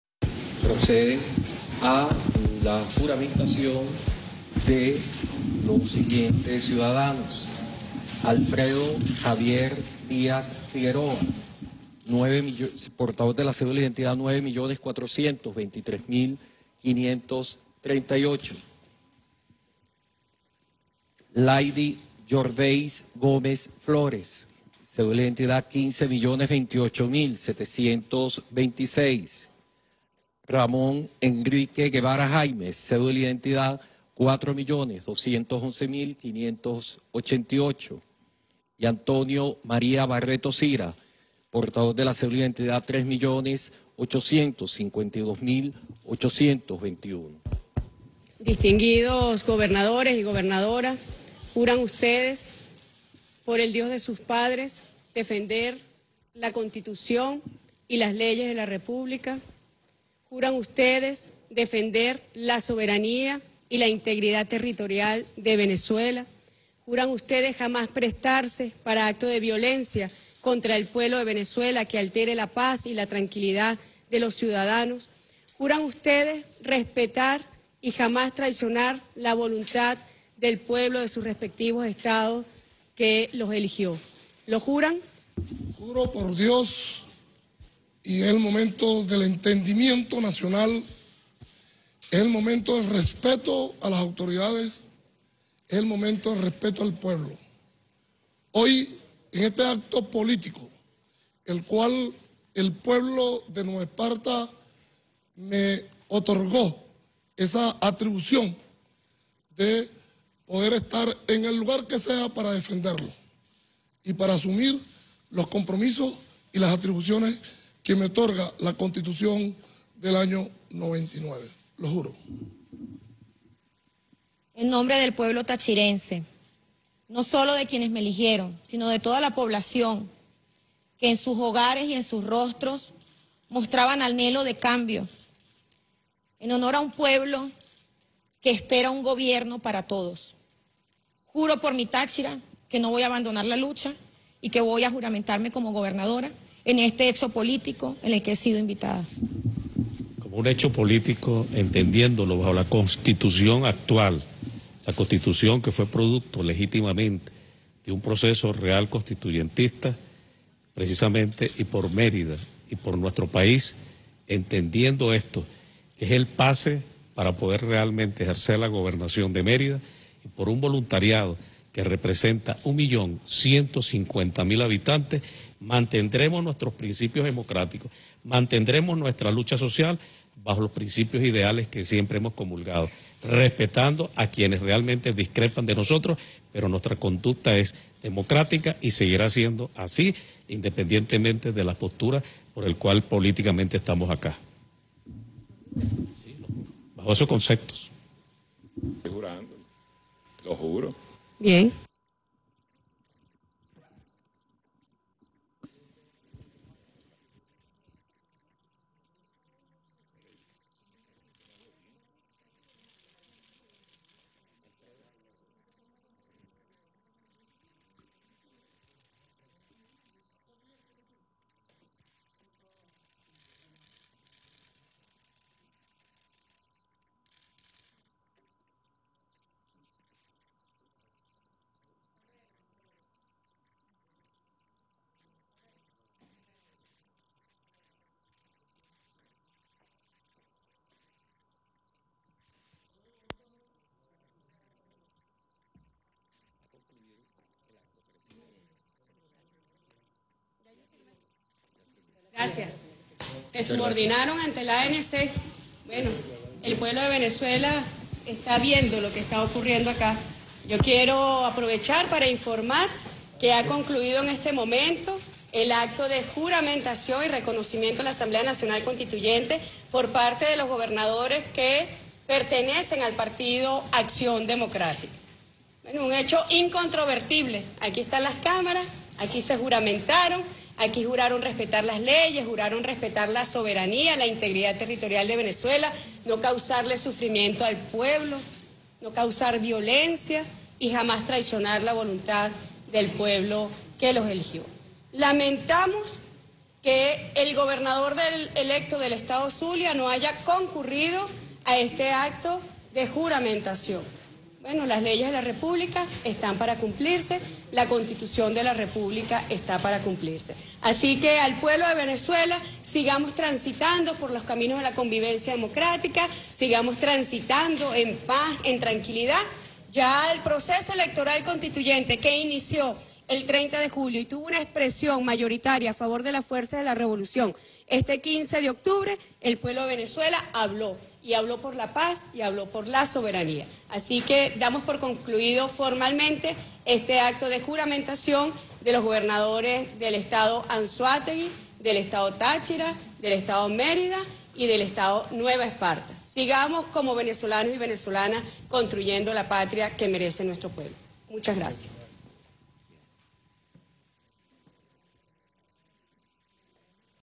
Juramentación de gobernadores opositores ante la Asamblea Constituyente, video completo
El pasado 23 de octubre de 2017, la gobernadora Laidy Gómez del estado Táchira y los gobernadores Antonio Barreto Sira de Anzoátegui,  Alfredo Díaz de Nueva Esparta y Ramón Guevara de Mérida, todos del partido Acción Democrática, se juramentaron ante la presidenta de la  Asamblea Nacional Constituyente, Delcy Rodríguez, y su junta directiva en Caracas, Venezuela. Video completo, con las palabras de Delcy Rodríguez y de cada uno de los gobernadores y gobernadora.